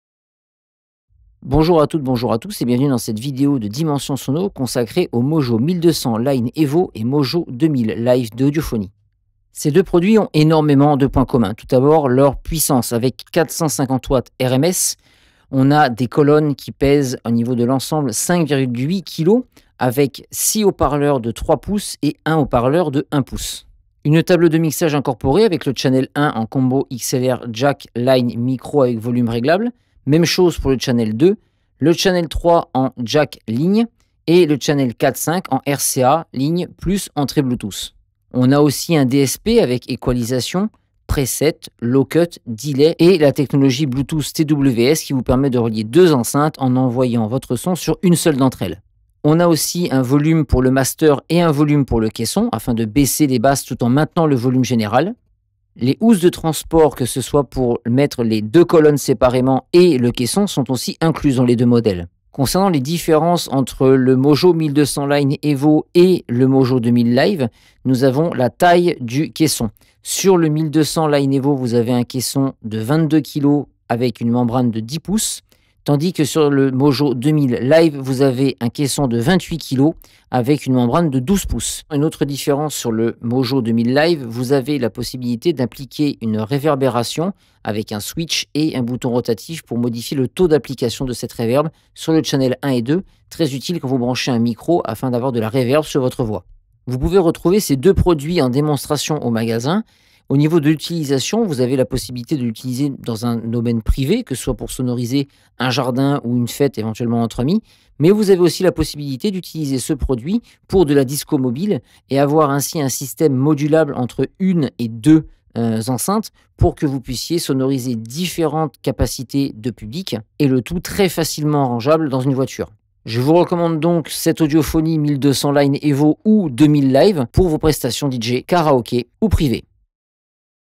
AUDIOPHONY - MOJO 1200 LINE EVO + MOJO 2000 LIVE
Dimension'Sono, magasin de vente, location, installation, Matériel Dj, Home Studio,  Sonorisation, Jeux de lumière, Vidéo à  Perpignan vous propose une vidéo explicative sur le MOJO 1200 Line Evo ainsi que le MOJO 2000 LIVE d'Audiophony